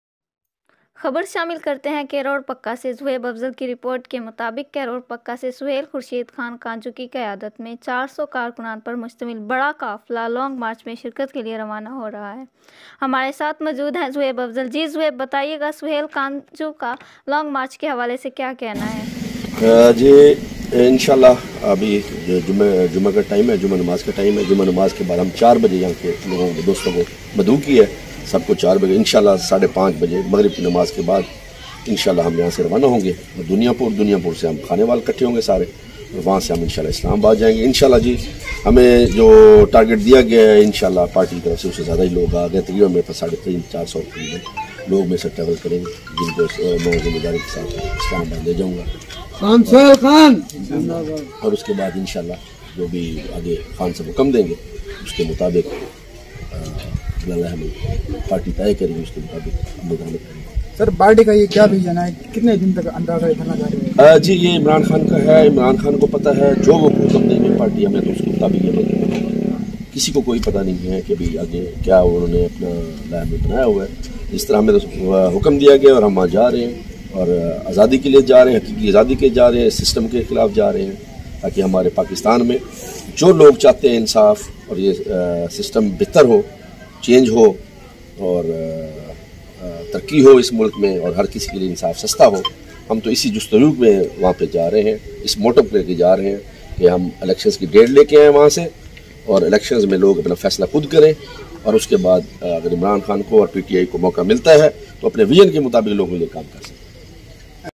PTI Sohail khan kanju Talks about long march
Sohail kanju Media se bat karty hoe